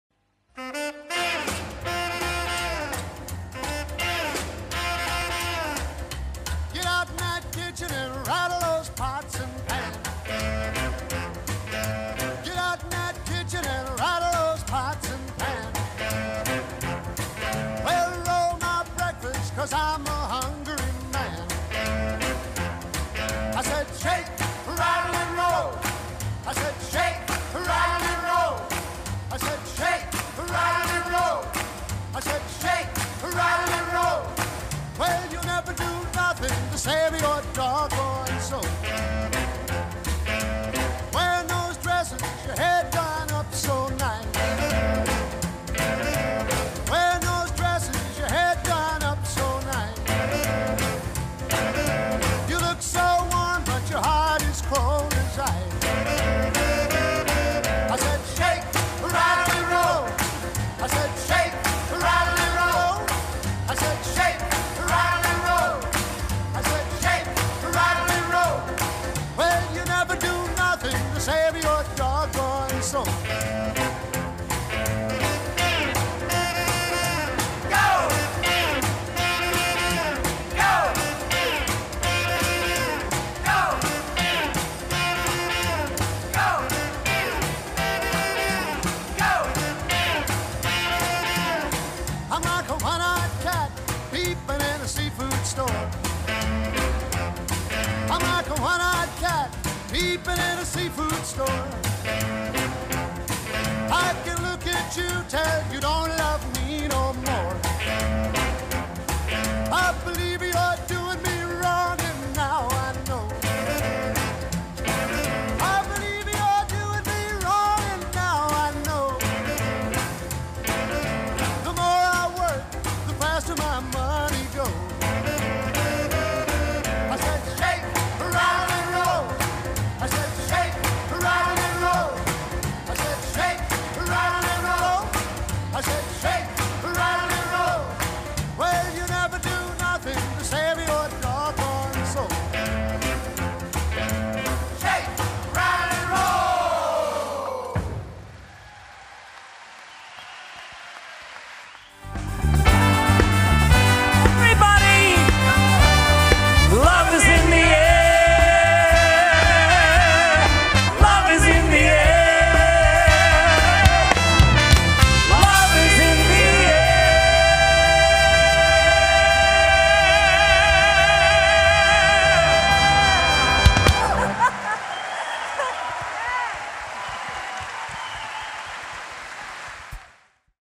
The Lord Mayor's Seniors Cabaret Gala 2019 - 05 Herb and Sonya
An afternoon with Brisbane’s gifted seniors and celebrity guests including Jackie Love, Normie Rowe and more in Brisbane City Hall.